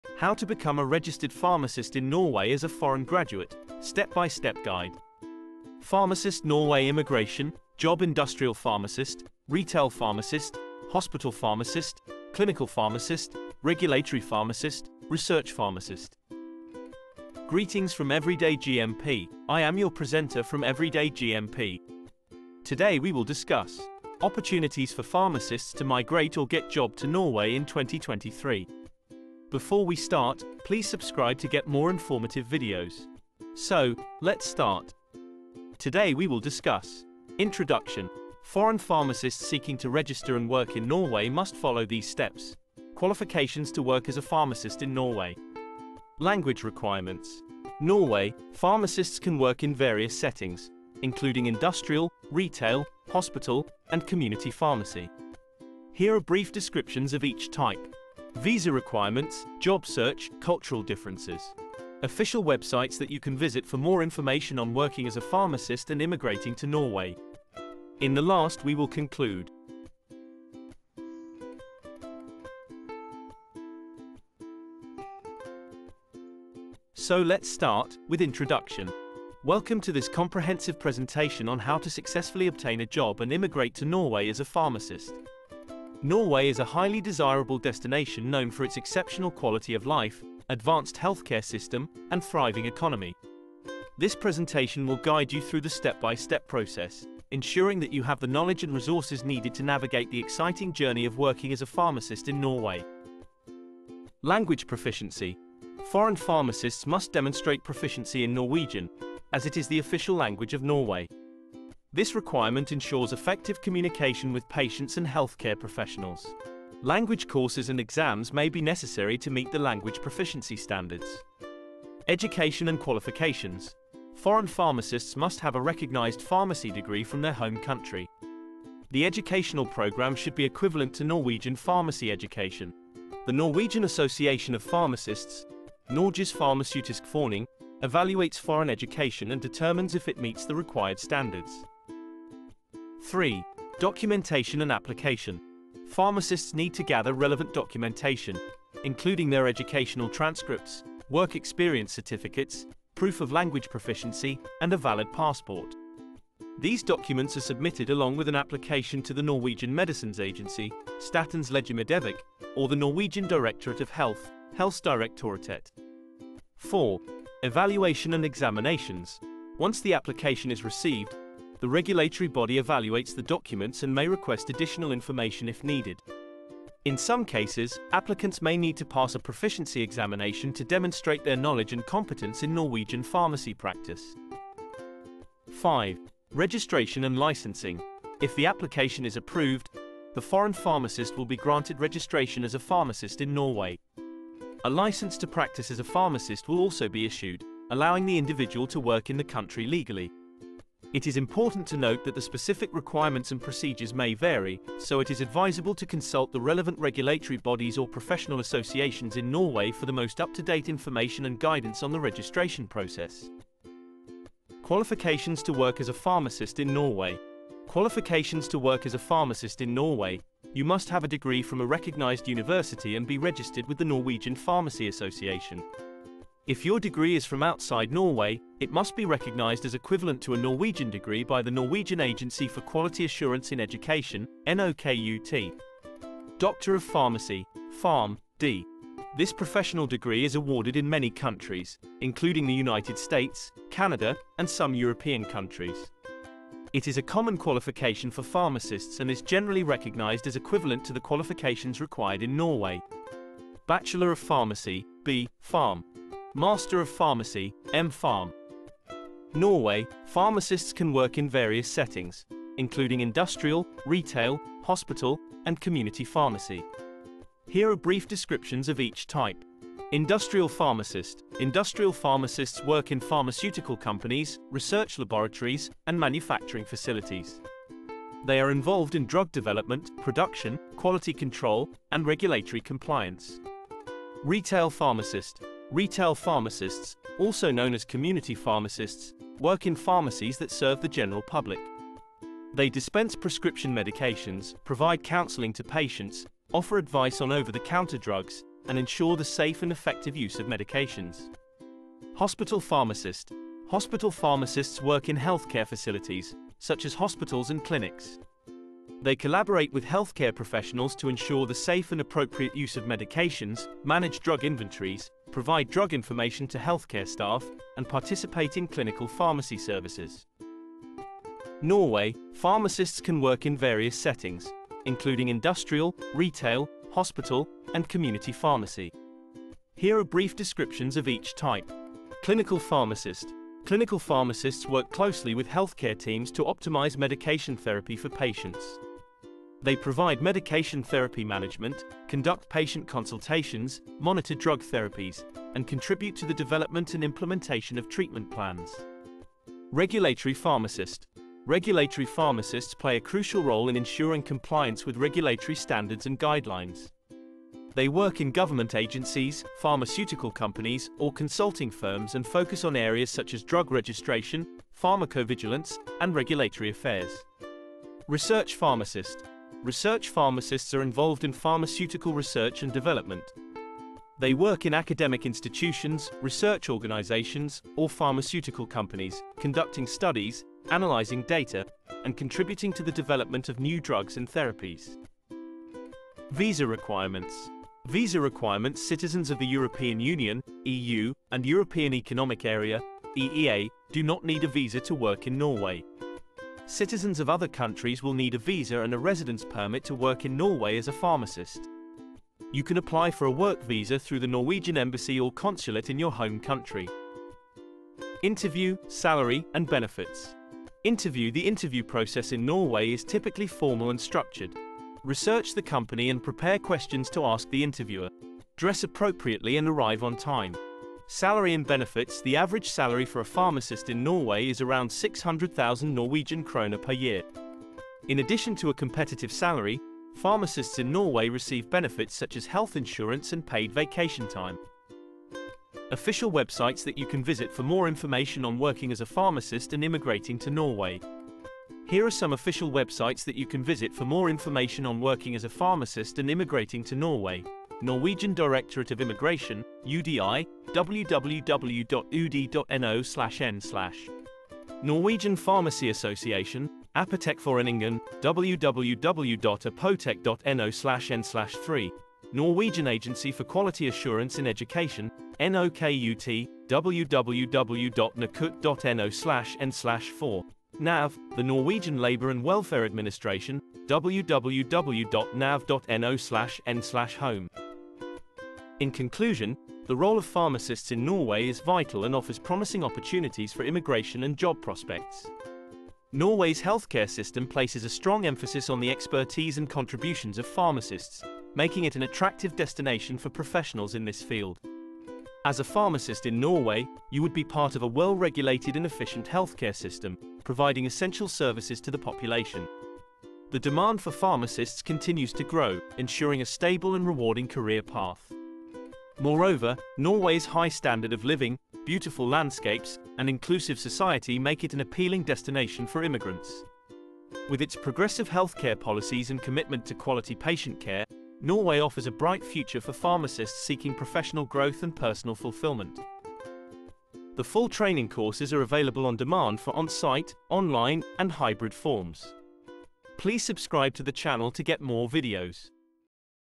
Norway | How to Become a Licensed Pharmacist in Norway as a Foreign Graduate | Step-by-Step Guide
Pharmacist in Norway | How to Become a Licensed Pharmacist in Norway as a Foreign Graduate | Step-by-Step GuideWelcome to this comprehensive presentation on how to successfully obtain a job and immigrate to Norway as a pharmacist. Norway is a highly desirable destination known for its exceptional quality of life, advanced healthcare system, and thriving economy. This presentation will guide you through the step-by-step process, ensuring that you have the knowledge and resources needed to navigate the exciting journey of working as a pharmacist in Norway.
In conclusion, the role of pharmacists in Norway is vital and offers promising opportunities for immigration and job prospects.
 Norway's healthcare system places a strong emphasis on the expertise and contributions of pharmacists, making it an attractive destination for professionals in this field.
As a pharmacist in Norway, you would be part of a well-regulated and efficient healthcare system, providing essential services to the population.
The demand for pharmacists continues to grow, ensuring a stable and rewarding career path.
Moreover, Norway's high standard of living, beautiful landscapes, and inclusive society make it an appealing destination for immigrants. With its progressive healthcare policies and commitment to quality patient care, Norway offers a bright future for pharmacists seeking professional growth and personal fulfillment.
4. The-Norwegian-Directorate-of-Health-approval-of-foreign-professional-qualifications/pharmacist-reseptar-prescriptionist/

Pharmacist  Norway | Registered Pharmacist in Norway as a Foreign Graduate | Step-by-Step Guide